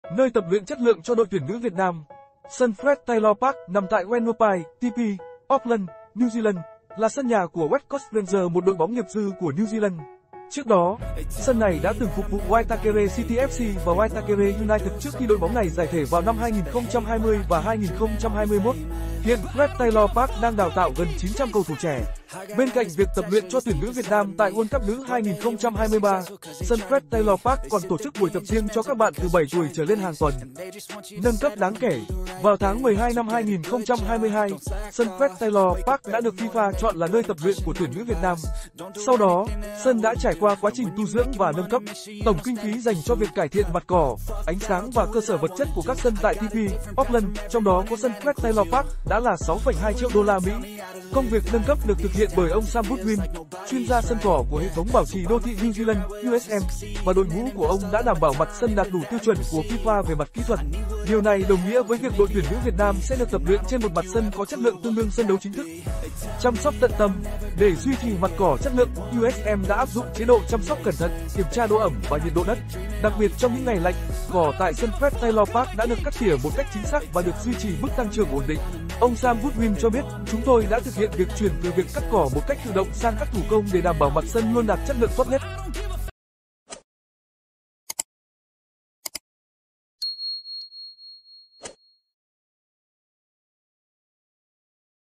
SÂN FRED TAYLOR PARK | NƠI TẬP LUYỆN CHẤT LƯỢNG CHO ĐỘI TUYỂN NỮ VIỆT NAM
Sân Fred Taylor Park, nằm tại Whenuapai, TP.Auckland (New Zealand), là sân nhà của West Coast Rangers - một đội bóng nghiệp dư của New Zealand. Trước đó, sân này đã từng phục vụ Waitakere City FC và Waitakere United trước khi đội bóng này giải thể vào năm 2020 và 2021. Hiện Fred Taylor Park đang đào tạo gần 900 cầu thủ trẻ. Bên cạnh việc tập luyện cho tuyển nữ Việt Nam tại World Cup nữ 2023, sân Fred Taylor Park còn tổ chức buổi tập riêng cho các bạn từ 7 tuổi trở lên hàng tuần.